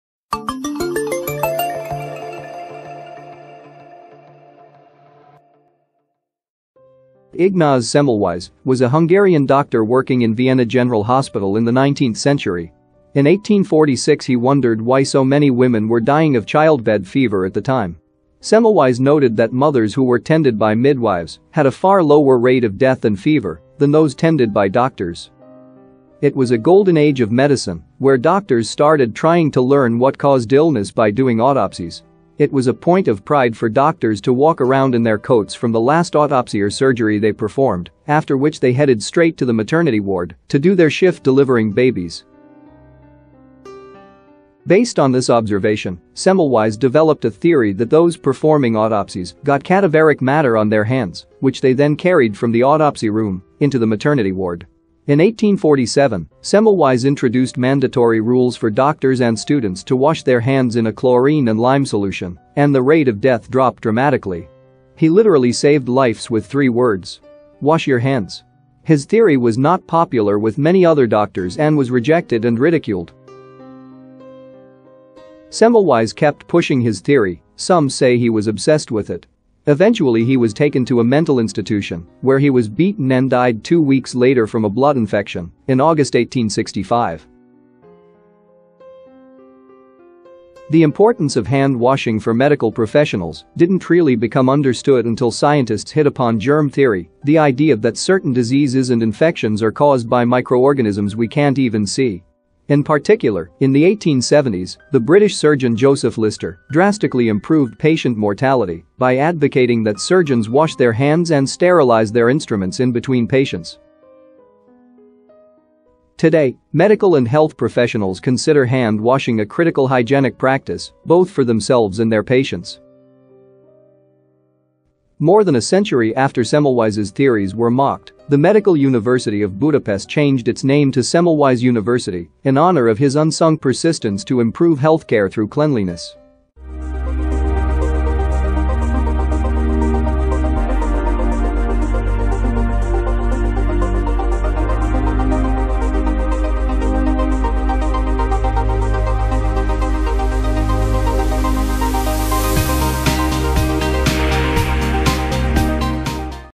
Pioneering Doctor Who Saved Lives With Three Words...
► This video is about a hungarian doctor who discovered the importance of hand washing. He's considered to be the father of 'hand hygiene' and 'infection control'. Brief history of the origin of hand washing.
► Everyone is singing songs and washing their hands these days, as if it is the most common thing in the world, but it wasn't always like this. In fact, even doctors and scientists thought it was useless, till not so long time ago.

Ignaz Semmelweis, was a hungarian doctor working in Vienna General Hospital and in 1846 he wondered why so many women were dying of 'childbed fever' at the time. Semmelweis noted that mothers who were tended by midwives had a far lower rate of death and fever than those tended by doctors.

It was a "golden age of medicine" where doctors started trying to learn what caused illness by doing autopsies. These started in Vienna hospitals in 1823. It was a point of pride for doctors to walk around in their coats from the last autopsy or surgery they performed, after which they headed straight to the maternity ward to do their shift delivering babies.

Based on this observation, Semmelweis developed a theory that those performing autopsies got cadaveric matter on their hands, which they then carried from the autopsy room into the maternity ward.

In 1847, Semmelweis introduced mandatory rules for doctors and students to wash their hands in a chlorine and lime solution, and the rate of death dropped dramatically.

The innovation was not popular with everyone: some doctors were disgruntled that Semmelweis was implying that they were to blame for the deaths and they stopped washing their hands, arguing in support of the prevailing notion at that time that water and miasmas were the potential cause of disease. In 1850s Semmelweis tried to persuade other doctors in European hospitals of the benefits of handwashing, but to no avail. His theory was rejected and ridiculed.

Semmelweis kept pushing his theory; some say he was obsessed with it. Eventually he was taken to a mental institution, where he was beaten and died two weeks later from a blood infection, in August 1865.

Sadly, the hand hygiene practices promoted by Semmelweis and later by Florence Nightingale were not widely adopted.

The importance of hand washing for medical professionals didn’t really become understood until scientists hit upon germ theory - the idea that certain diseases and infections are caused by microorganisms we can’t even see.

In particular, in the 1870s, the British surgeon Joseph Lister drastically improved patient mortality by advocating that surgeons wash their hands and sterilize their instruments in between patients.

But, in general, handwashing promotion stood still for over a century and did not become universal until relatively recently. It was not until the 1980s, that the United States Centers for Disease Control and Prevention identified hand hygiene as an important way to prevent the spread of infection.

Today, medical and health professionals consider hand washing a critical hygienic practice, both for themselves and their patients.

Washing your hands with soap is one of the most important and cost-effective things you can do to help prevent and control the spread of many illnesses, thus saving lives too.

More than a century after Semmelweis’s theories were mocked, the Medical University of Budapest changed its name to Semmelweis University, in honour of his unsung persistence to improve healthcare through cleanliness.